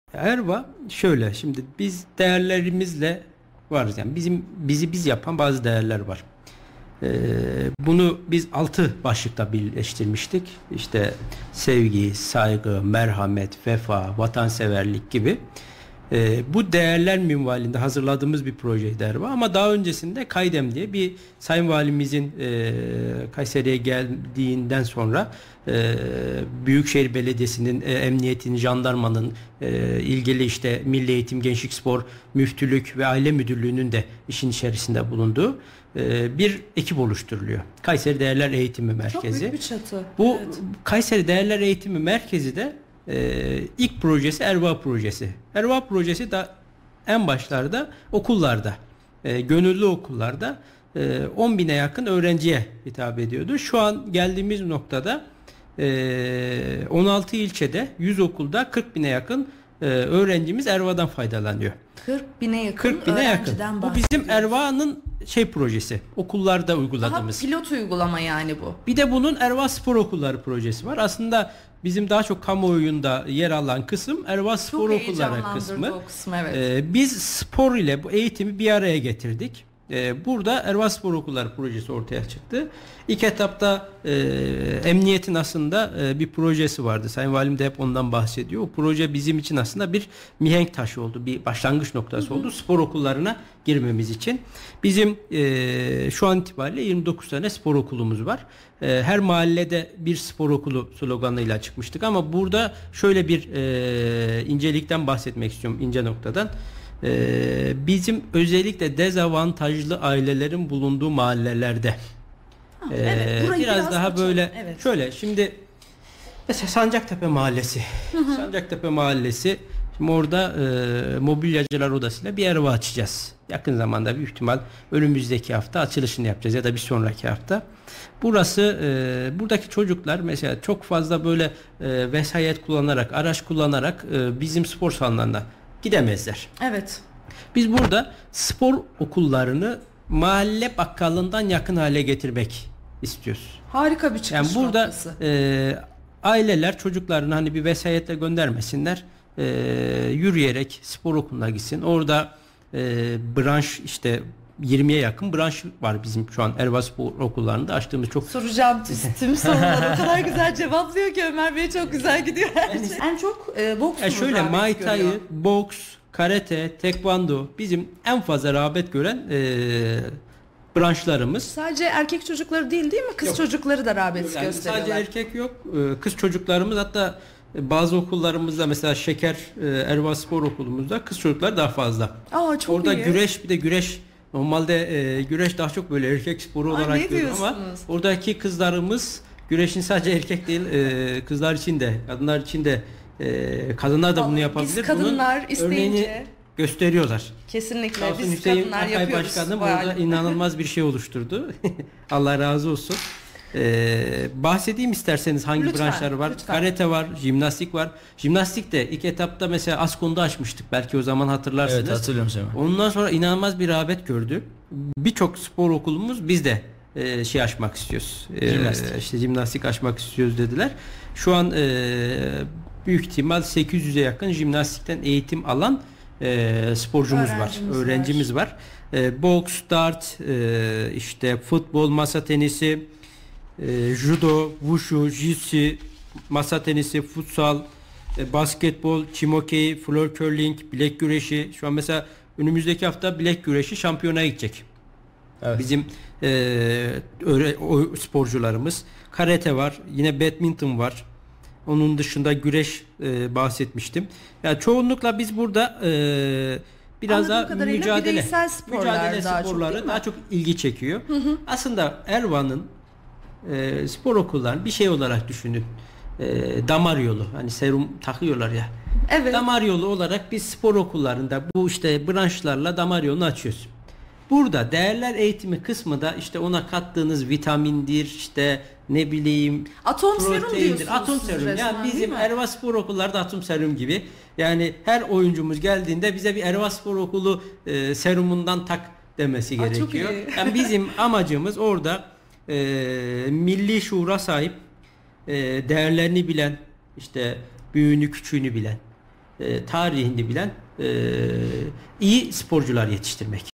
Vali Yardımcısı Tekeş, “ERVA Spor Okullarını mahalle bakkalından yakın hale getirmek istiyoruz”
Kayseri Vali Yardımcısı Ömer Tekeş, ERVA Spor Okullarına ulaşımın kolay olmasının amaçlandığını vurgulayarak, “Her mahallede bir spor okulu sloganıyla çıkmıştık ama burada dezavantajlı ailelerin bulunduğu mahallelerde okullar var. Sancaktepe Mahallesinde mobilyacılar odası ile bir okul daha açılacak. Buradaki çocuklar araç kullanarak gidemezler bu yüzden spor okullarını mahalle bakkalından yakın hale getirmek istiyoruz. Aileler çocuklarını kolayca gönderebilsinler” dedi.
Haberin detaylarına haber sitemizden ulaşabilirsiniz.